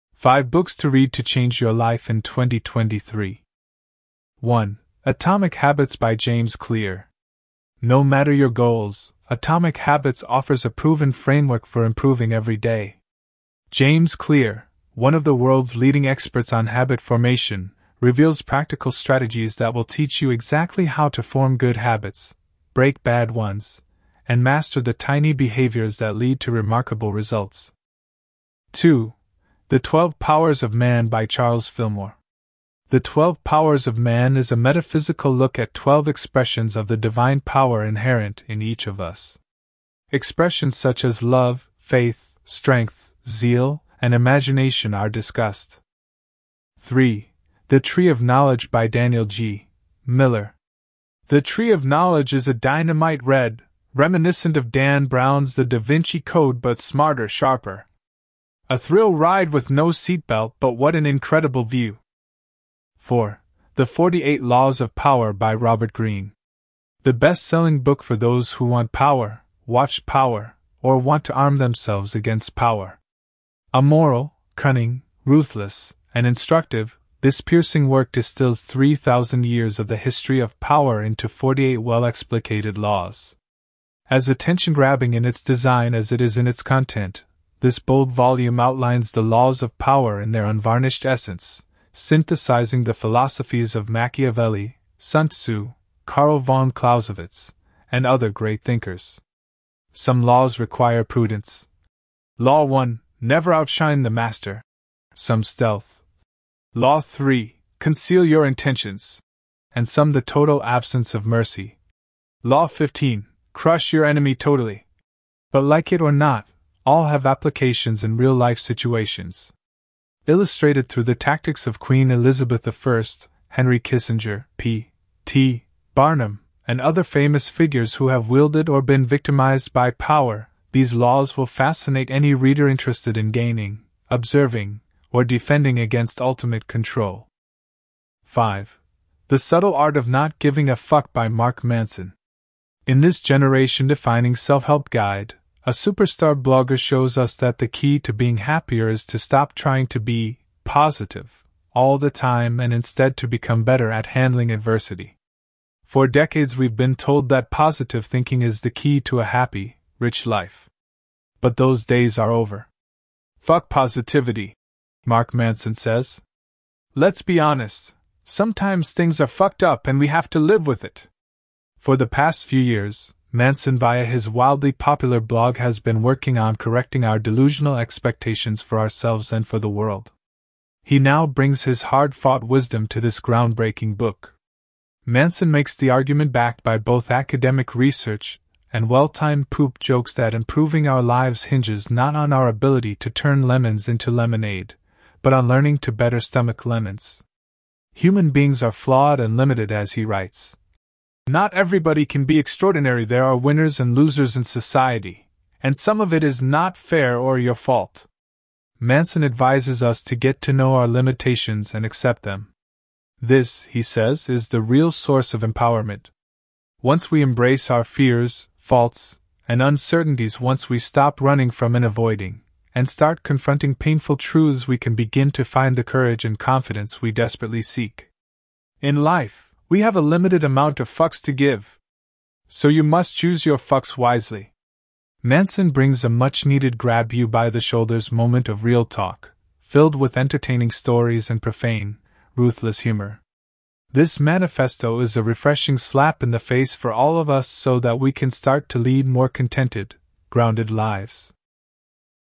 "Unlock Your Potential: 5 Books That Can Transform Your Life in 2023!"
Are you ready to take your life to the next level? In this video, I'm sharing with you 5 books that can help you unlock your potential and transform your life in 2023!

These 5 books will help you overcome any obstacle in your path and help you reach your goals in life. Whether you're looking to quit your job, start a new business, or achieve your dream career, these books have you covered! So what are you waiting for? Let's start reading!

Books in the video:
Atomic Habits

The Twelve Powers

The Tree of Knowledge

The Subtle Art of Not Giving a F*ck

The 48 Laws of Power

It works: The Famous Little Red Book That Makes Your Makes Your Dreams Come True by Roy Herbert Jarrett